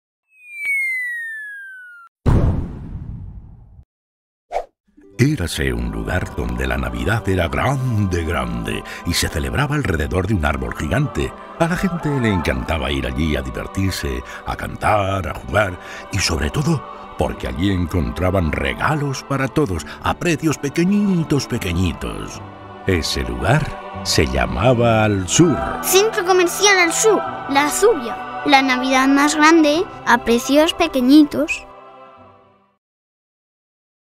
La navidad más grande a precios pequeñitos | Babydog Comunicación
Cuña de radio para el centro comercial ALHSUR resaltando las precios tan bajos que ofrecen.